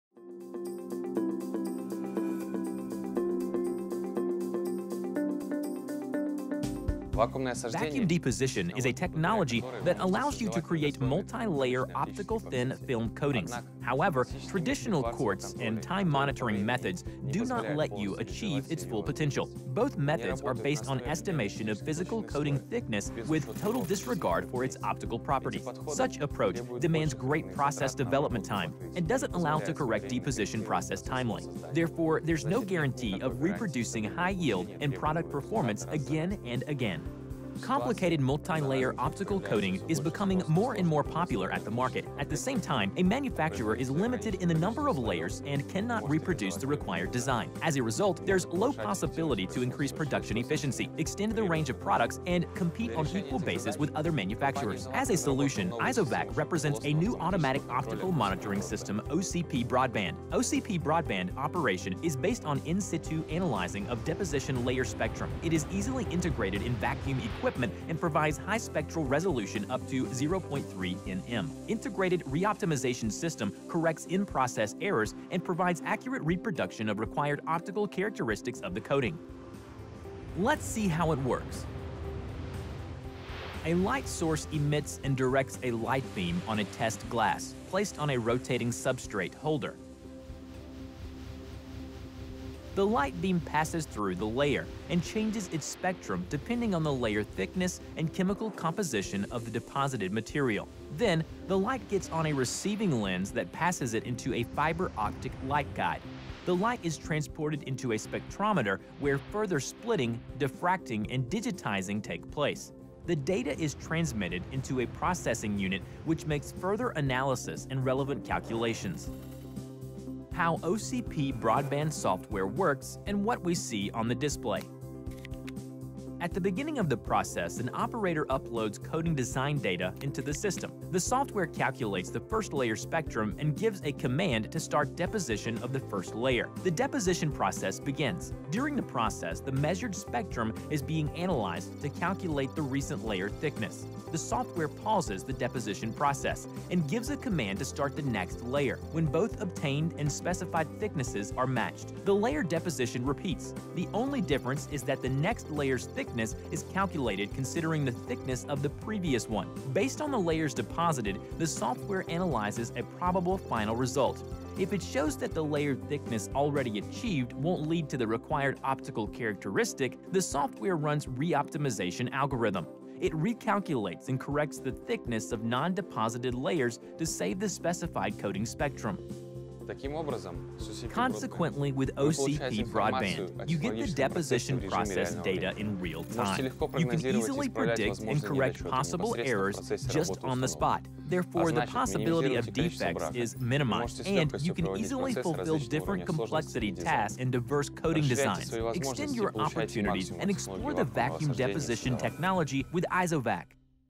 OCP Broadband - IZOVAC Group
OCP (Optical Coating Provider) Broadband is easily integrated hardware and software solution which provides fully automatic control over all types of deposition of optical coatings for different vacuum equipment in wide optical range with spectral resolution up to 0.3nm.
Primary deposition process control mode is direct transmission through substrate placed on rotatable substrate holder. Spectrometer measures real coating spectrum throughout the working range after each revolution of substrate holder. Software algorithm analyzes measured spectrum based on the optical design of required coating and computes breakpoint for each layer. Close integration with various PLC and Deposition Controllers of vacuum equipment makes it possible to realize optical deposition process automatically from the beginning to the end. Moreover, to obtain high level quality and manufacturing yield of the desired coating OCP Broadband performs design optimization for all remaining layers and applies changes online if necessary.